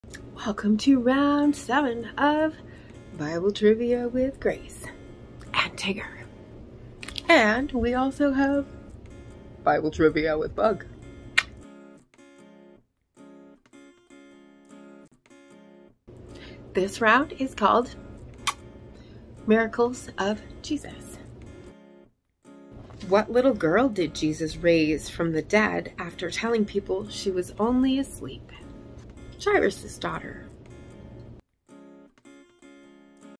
Bible Trivia Round 7 Answer #20
Round 7  "Miracles of Jesus"
in celebration of the Christmas season
Now Daily Questions and answers!

Bible Trivia fun with Metamorphic Grace from the book
The Complete Book of Bible Trivia by J. Stephen Lang

answers to trivia questions are given the next day